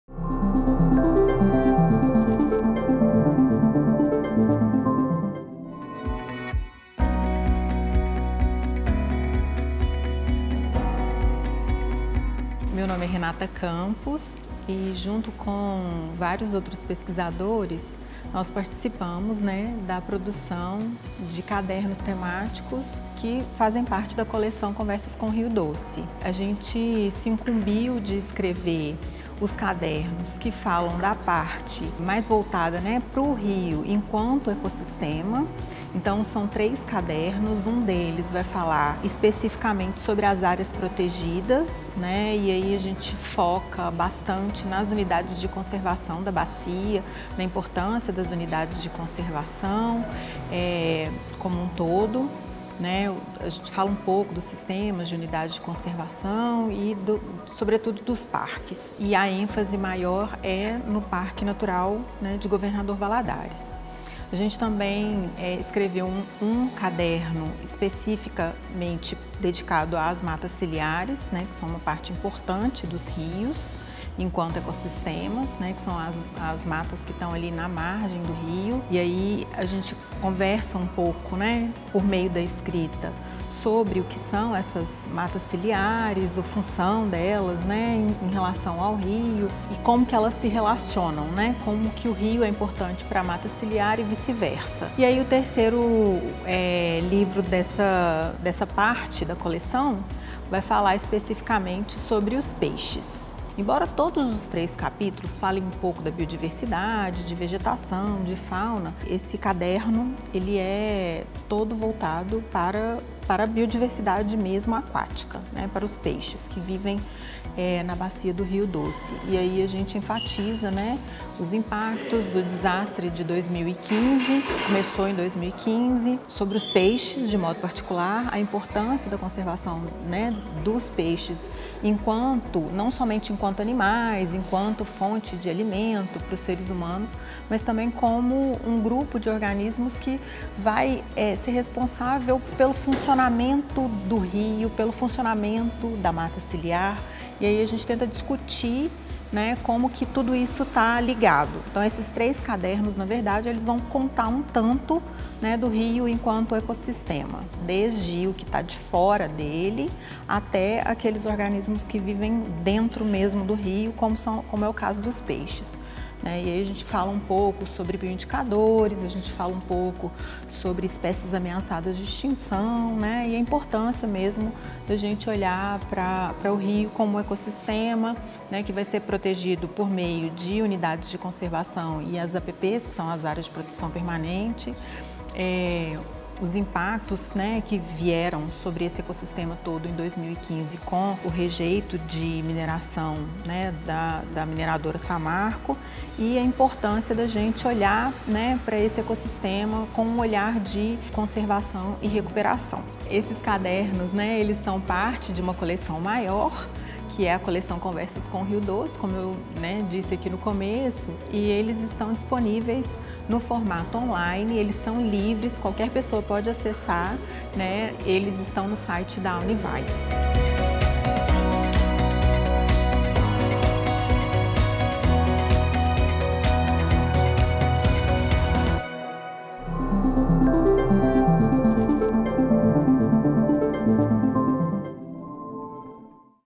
Nossos Escritores | Renata Campos - Cadernos Temáticos
No quadro "Nossos Escritores", a professora da Univale, Renata Campos,  apresenta sua participação em três cadernos temáticos da coleção "Conversas com o Rio Doce".

No primeiro caderno, com foco em Áreas Protegidas e Unidades de Conservação, ela escreve sobre a importância da proteção do meio ambiente e da biodiversidade da bacia do Rio Doce.

Já no segundo caderno, dedicado às Matas Ciliares da Bacia do Rio Doce: Impactos do Rompimento da Barragem de Fundão, ela aborda os impactos do rompimento da barragem de Fundão na mata ciliar do Rio Doce.

E no terceiro, sua abordagem é sobre os "Peixes da Bacia do Rio Doce: Diversidade e Principais Ameaças". Nele, a professora escreve sobre a diversidade e as principais ameaças aos peixes que vivem na bacia do rio Doce.